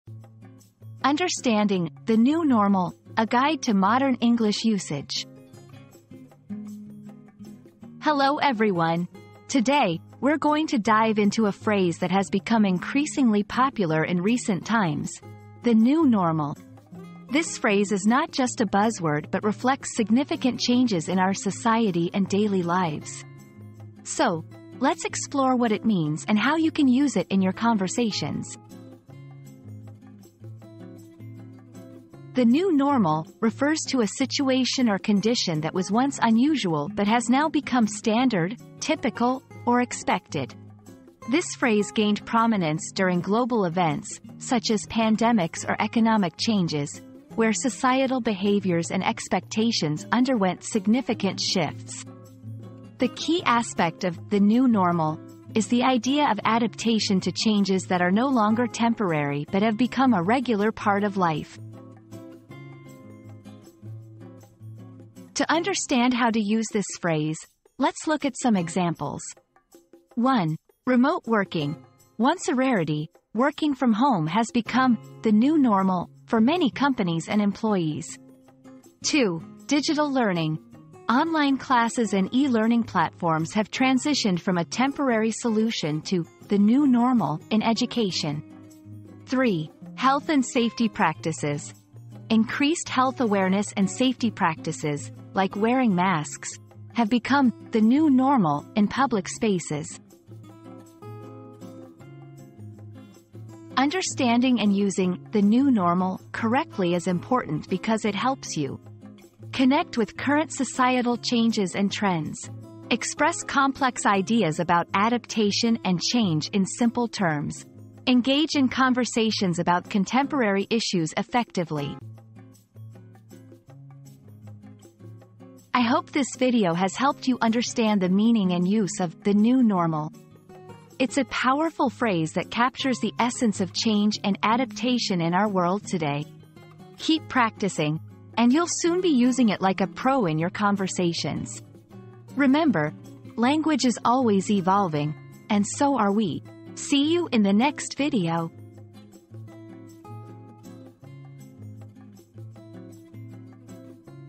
Understanding "The New Normal": A Guide to Modern English Usage
00:00 • Introduction - Understanding "The New Normal": A Guide to Modern English Usage
00:31 • What Does "The New Normal" Mean?
01:04 • Examples of "The New Normal" in Context
01:43 • Why is Understanding "The New Normal" Important?